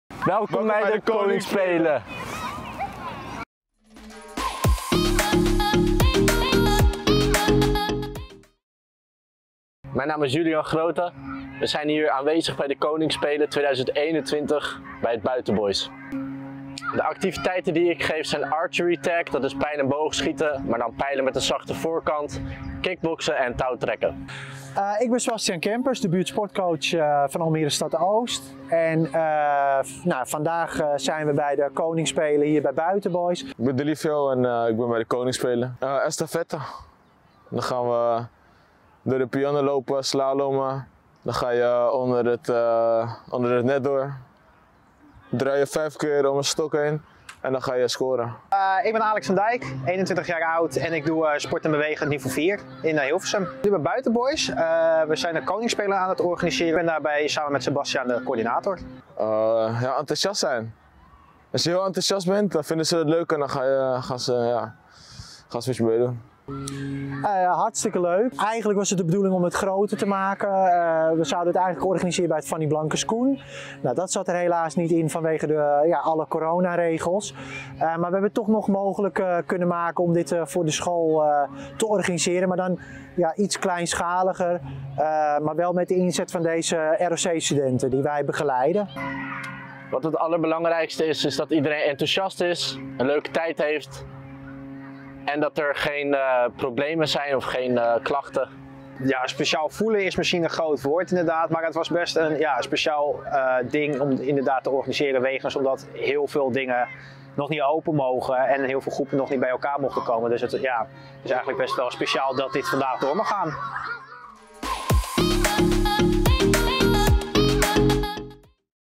Koningsspelen 2021
Het is weer Koningsdag en traditie getrouw zijn er de dag ervoor de koningsspelen.

Daarnaast wensen wij de koning een fijne verjaardag toe!